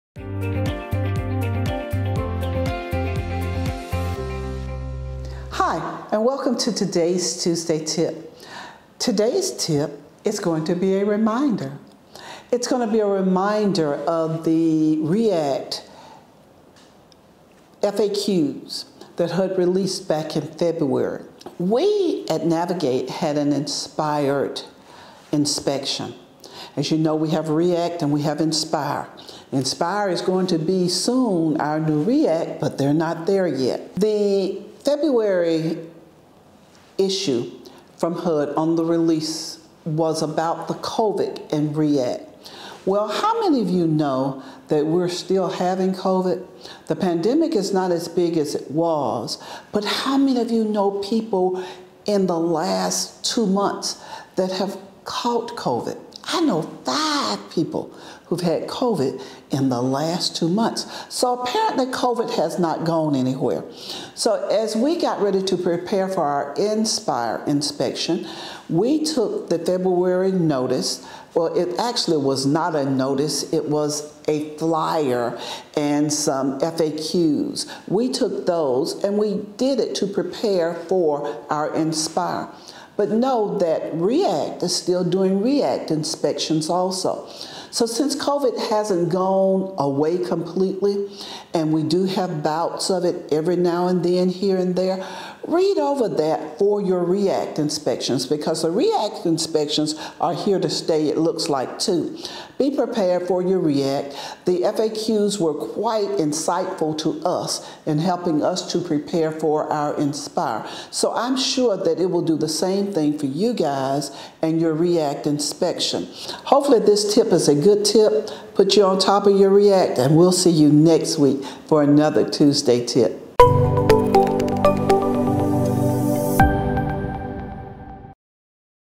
Tuesday Tip: REAC FAQ Reminder
In this week's TuesdayTip, why we are encouraging everyone to read over the REAC FAQ HUD released this spring!